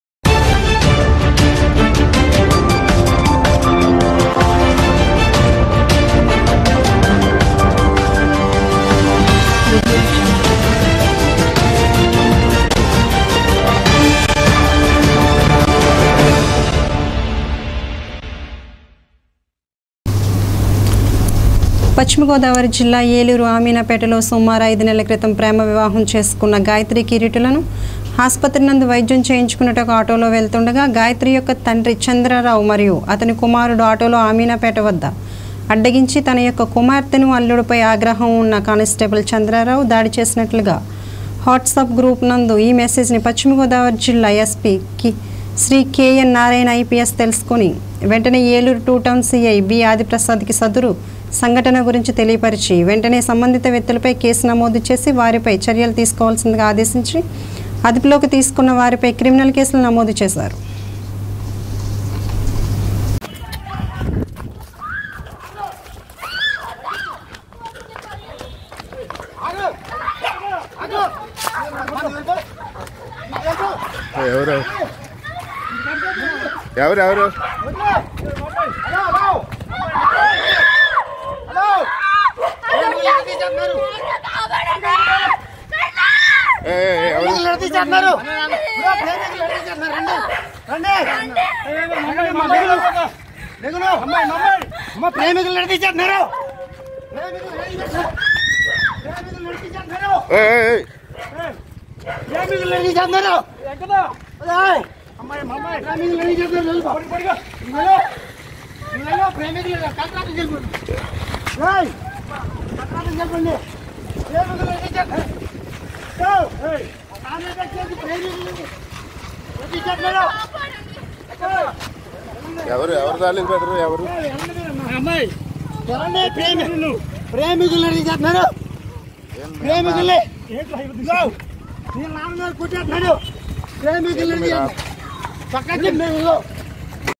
కానిస్టేబుల్ చంద్ర రావు దాడి చేసినట్లుగా వాట్స్అప్ గ్రూపు మెసేజ్ ని పశ్చిమ గోదావరి జిల్లా ఎస్పీ
కానిస్టేబుల్ చంద్ర రావు దాడి చేసినట్లుగా వాట్స్అప్ గ్రూపు నందు ఈ మెసేజ్ ని పశ్చిమ గోదావరి జిల్లా ఎస్పీ శ్రీ కే.ఎన్.నారాయణ్ ఐపీఎస్